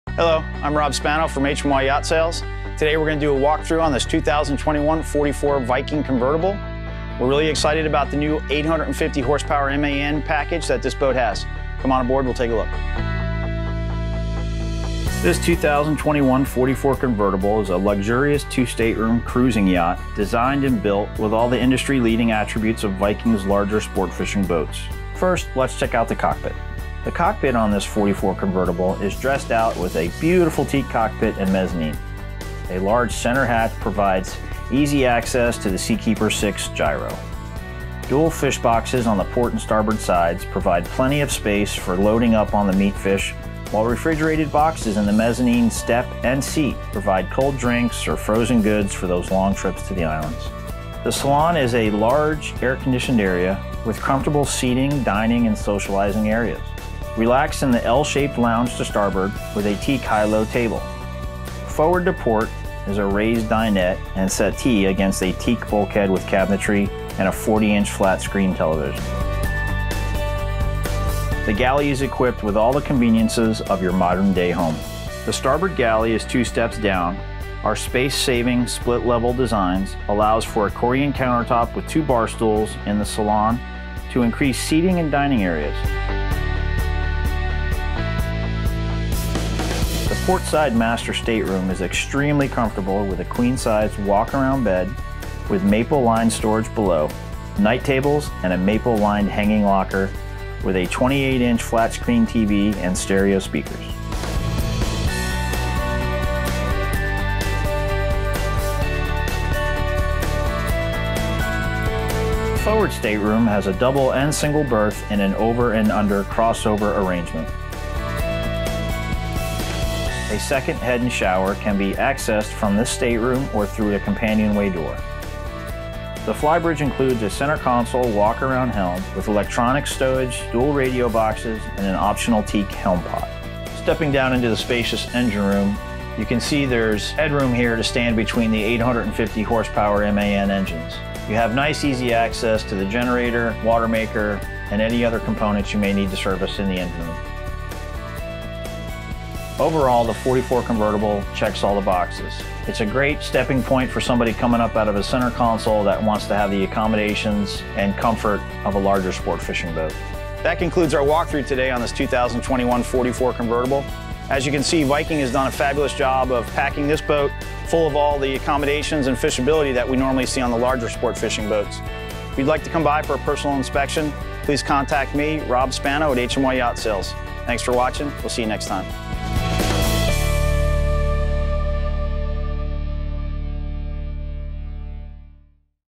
2021 Viking 44' Convertible [Walkthrough]
For more information on the New Viking Yachts 44' Convertible, for Sale with HMY Yachts,

The Viking Yachts 44 Convertible is a mid-size fishing machine designed specifically for customers who find this amount of boat to be fitting for their space, layout and performance needs. If the 48 convertible is a little too much boat for you and the 42 Convertible isn’t quite enough, the Viking 44 Convertible should do the trick. The Viking 44′ has taken a page from the book of her successful predecessor, the 42 Convertible, implementing a handful of the 42’s most ordered features. Some of the highlights you can expect to find on board the 44 Convertible include standard power or MAN I6 CRM: 800 MHPs, oil change system for engines, generator and transmissions, generator upgrade to Onan 13.5 kw, a mid-ship master stateroom, mid-ship master stateroom, sheets for beds, polished Corian countertops, upgraded helm chairs, cockpit freezer, drink box, livewell in transom box and a fuel capacity of 845 gallons w/ fuel transfer system.